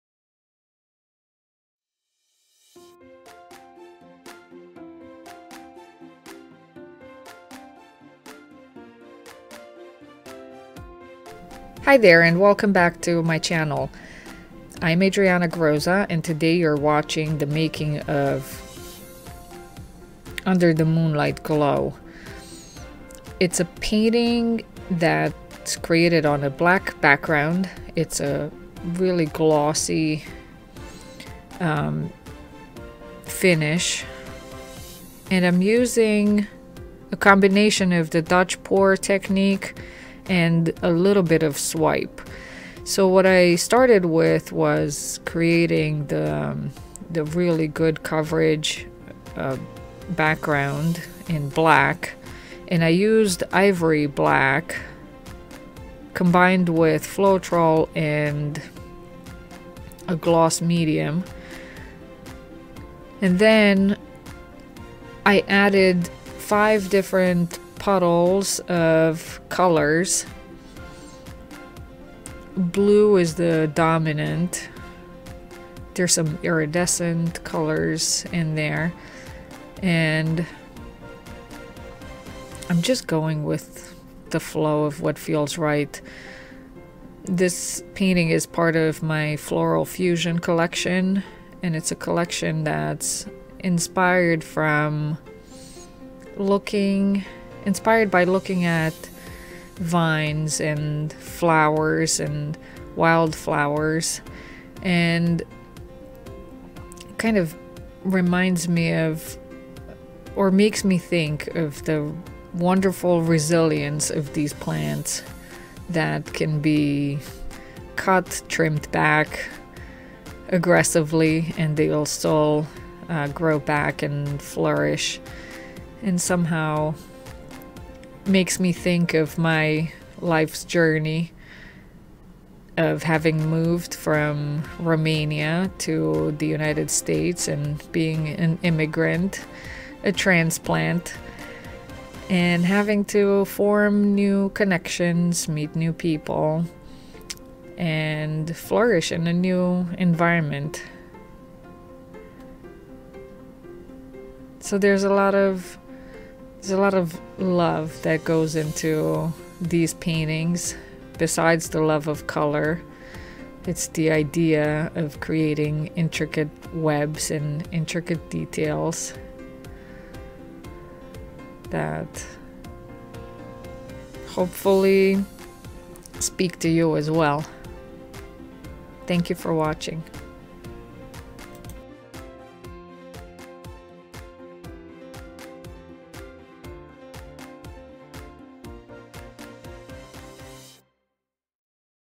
Under The Moonlight Glow - Making of- Black background- Large Canvas- Fluid acrylics-
"The Floral Fusion series explores the complex relationship between the familiar and the unknown, the reality and the imagination. As an artist, I have always had a passion for exploring the depths of color and composition. In this collection, I sought to create works of art that defy traditional boundaries and tools, invoking a sense of intrigue and discovery through vibrant colors and bold movements.
The inspiration for this collection comes from the resilience of weeds and their flowers. As they grow and reach out in all directions, constantly weaving intricate webs and forging new connections with their surroundings, they inspire me to see the hidden beauty of everyday life. As an immigrant from Romania, I view my own life as that of a weed, where I have developed new roots, formed new connections, relationships, and my own social web."

Materials used:
Liquitex, Golden Paints mixed with Floetrol and a US Supply gloss medium, plus water- approximate ratio 1:6:1

Colors used:
Ivory Black
Irridescent blue
Ultramarine blue
Gold
Dioxazine purple
Teal blue
Turquoise
Permanent red
Pearlized Green
Pearlized Blue